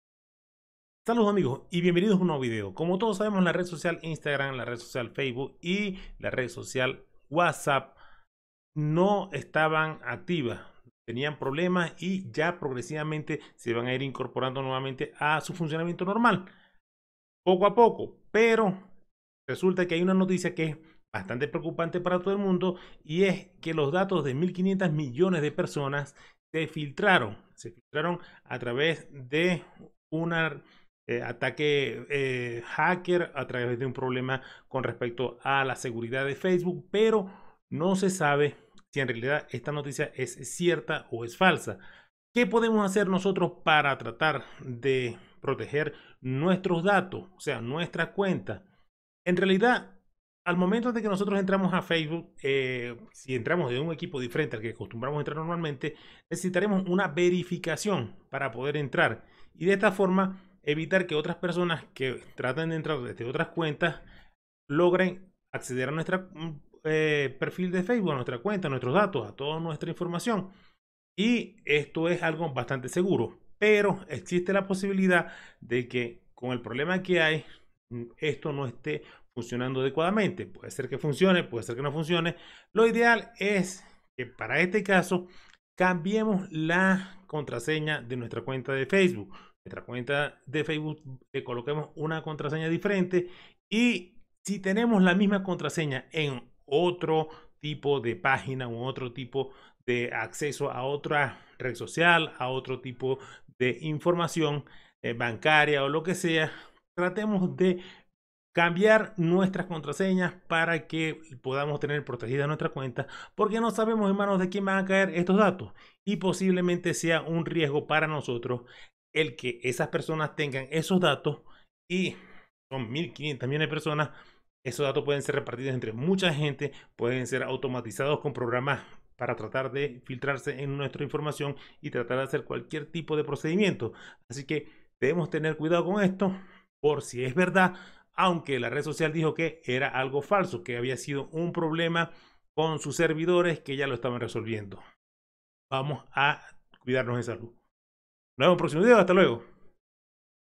Se filtran datos de usuarios de facebook
Se filtran datos de usuarios de facebook los datos de mas de 1500 millones de personas han sido filtrados.